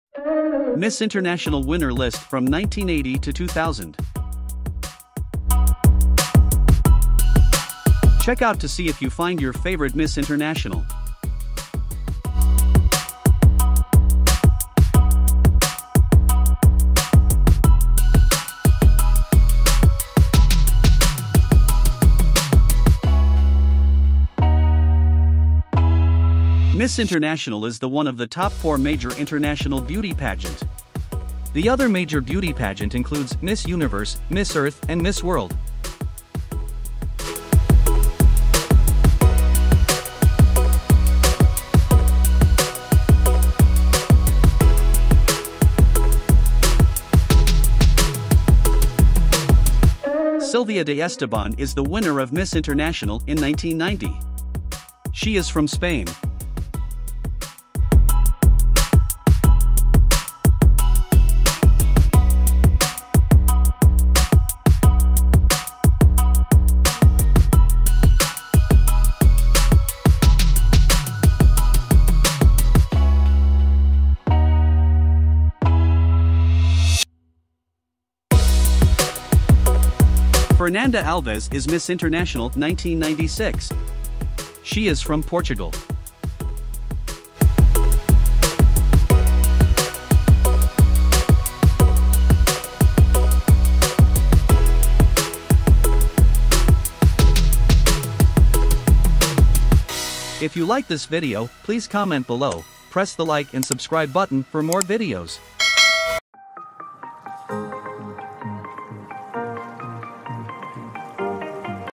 Miss International Winner 1980 to 2000
Miss International Winner List from 1980 to 2000. Check out to see if you find your favorite Miss International.

Miss International is the one of the top 4 major international beauty pageant. The other major beauty pageant includes Miss Universe, Miss Earth and Miss World.

Silvia de Esteban is the winner of Miss International in 1990. She is from Spain.

Fernanda Alves is miss international 1996. She is from Portugal.
0:00 Miss International 1980
0:49 Miss International 1990
1:39 Miss International 2000

Recent Winners:
Miss International 2022 - Jasmin Selberg - ‎Germany

the list of Miss International Winner List from 1980 to 2000 in alphabetically: